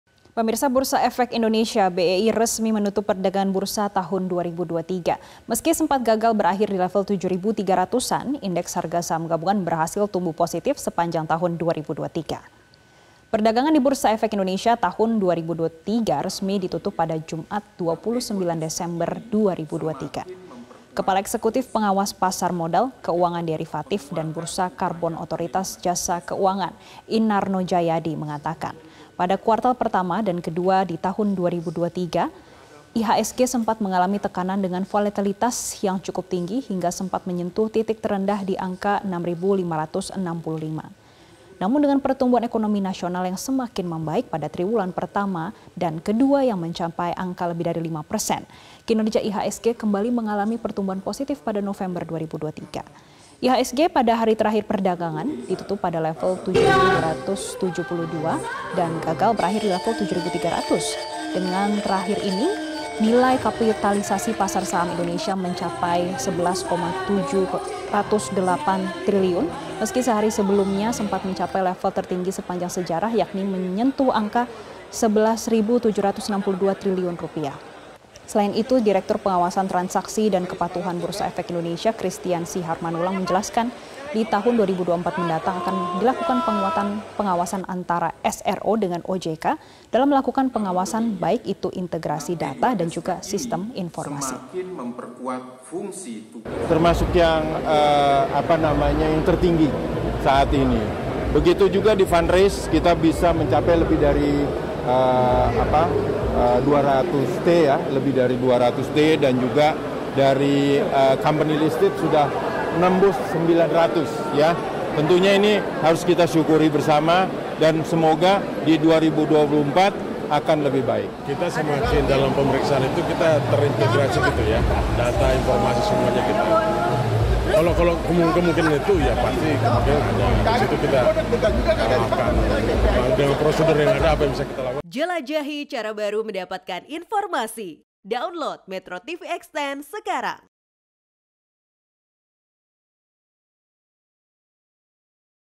Dibayang Resesi, IHSG Berhasil Tumbuh Positif Sepanjang 2023
MetroTV, Bursa Efek Indonesia (BEI) resmi menutup perdagangan bursa tahun 2023. Meski sempat gagal berakhir di level 7.300-an, indeks harga saham gabungan berhasil tumbuh positif sepanjang tahun 2023.